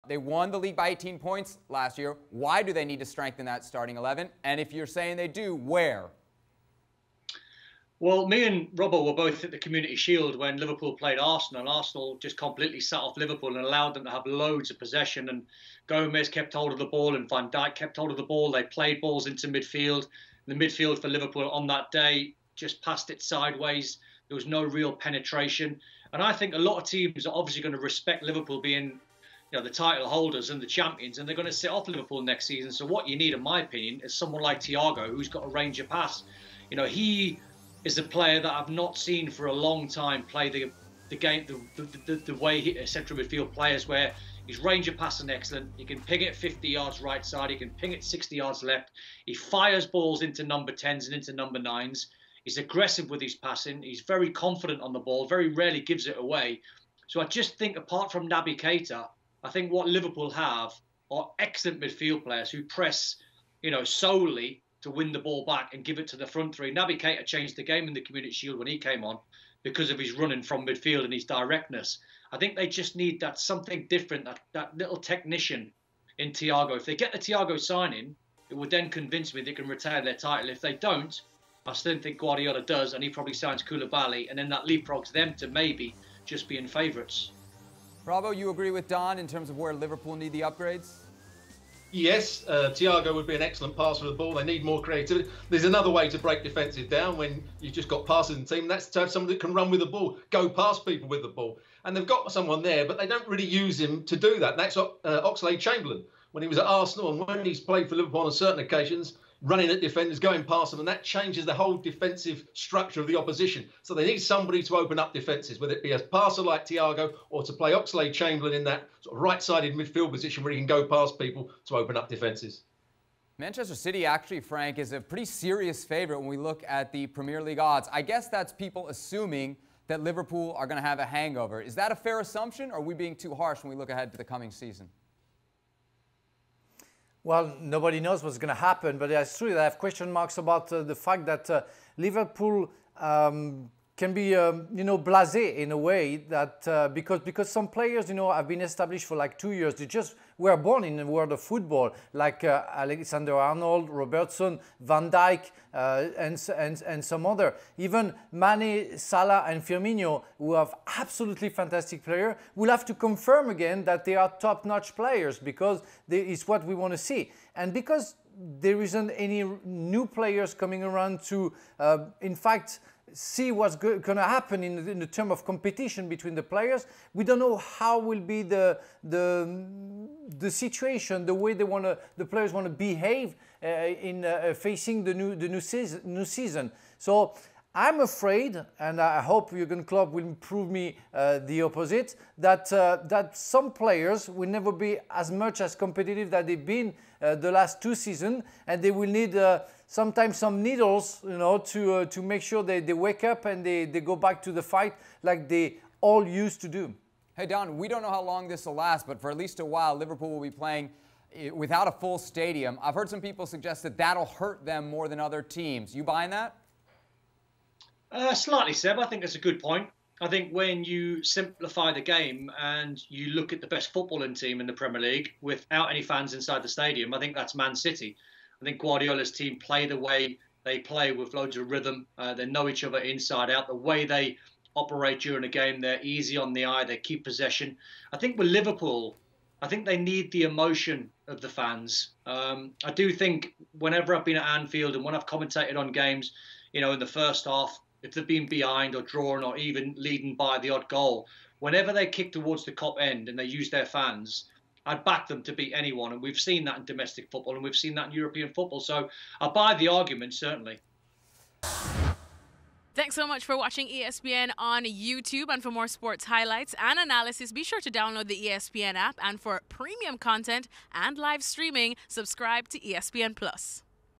Liverpool season preview: Why an empty Anfield could hinder the Reds’ title defence | Premier League
Despite winning the Premier League title last season by 18 points, it’s Manchester City, not Liverpool, that are favourites to win it all this year. ESPN FC’s Don Hutchison, Stewart Robson and Frank Leboeuf discuss Jurgen Klopp’s lack of transfer activity this summer, and suggest why continuing to play in front of no fans at Anfield could prove to be a disadvantage.